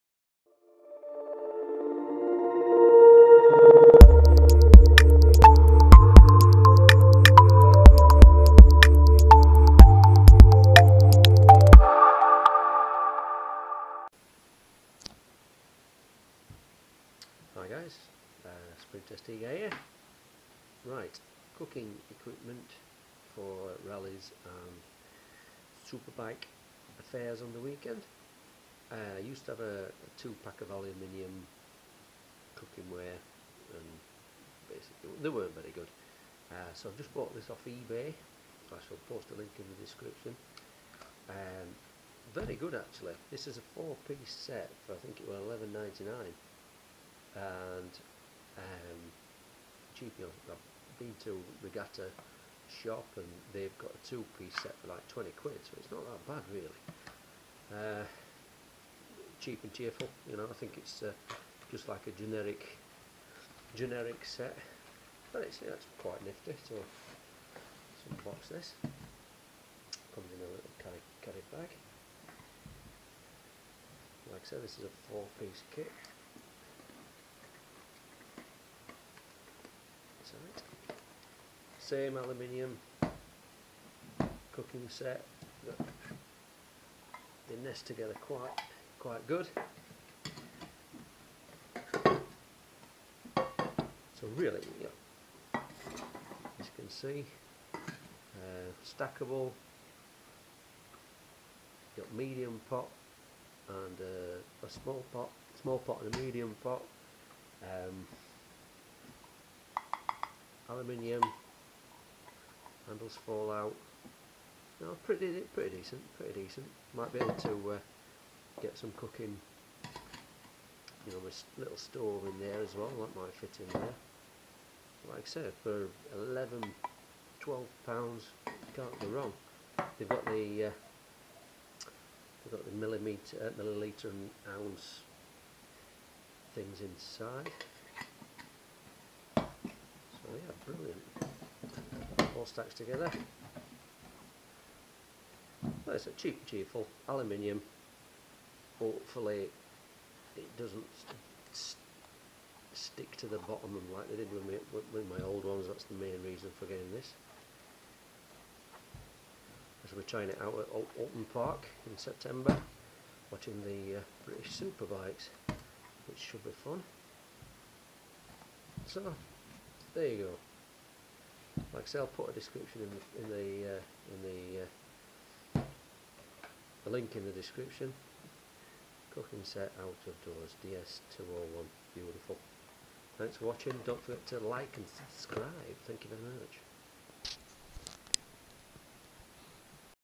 New Camping cookwear
A four pot aluminium cookwear set from ebay